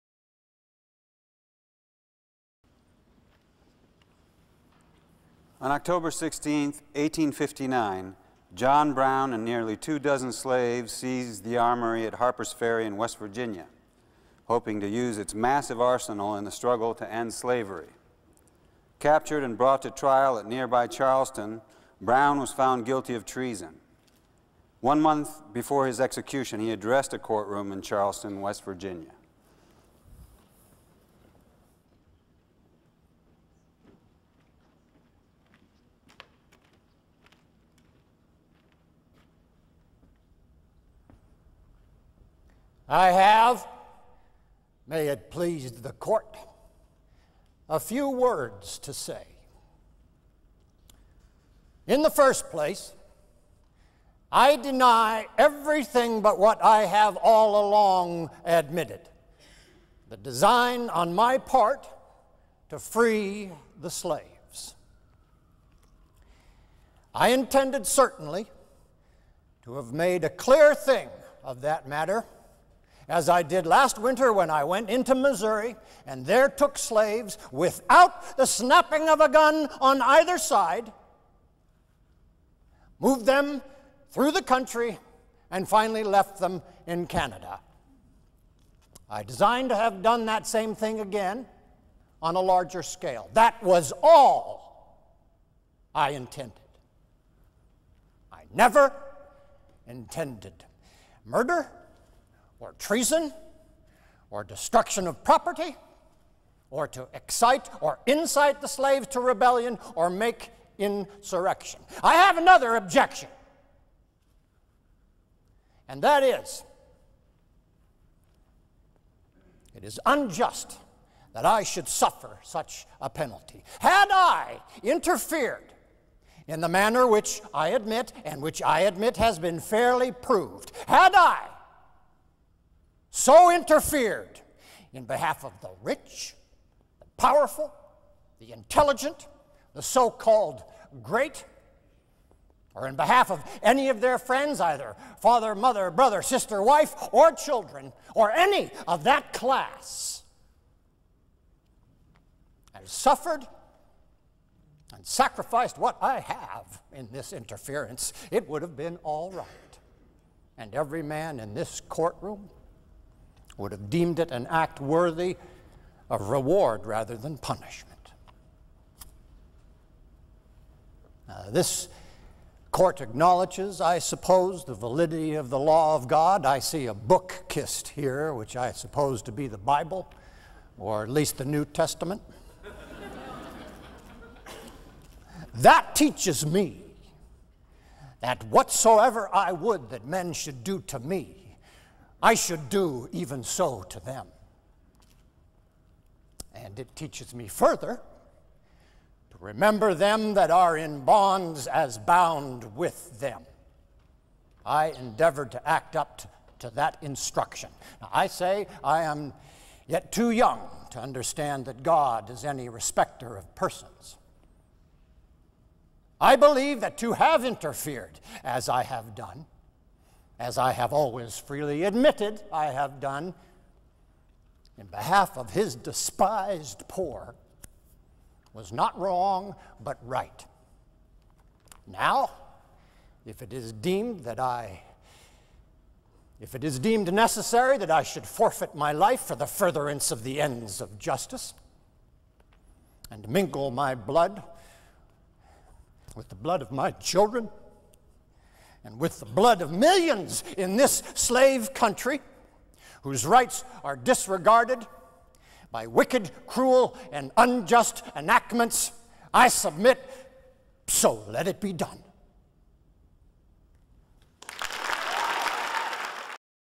John Brown's last speech
Recited by David Strathairn

John Brown was a man of action -- a man who would not be deterred from his mission of abolishing slavery. On October 16, 1859, he led 21 men on a raid of the federal arsenal at Harpers Ferry, Virginia. His plan to arm slaves with the weapons he and his men seized from the arsenal was thwarted, however, by local farmers, militiamen, and Marines led by Robert E. Lee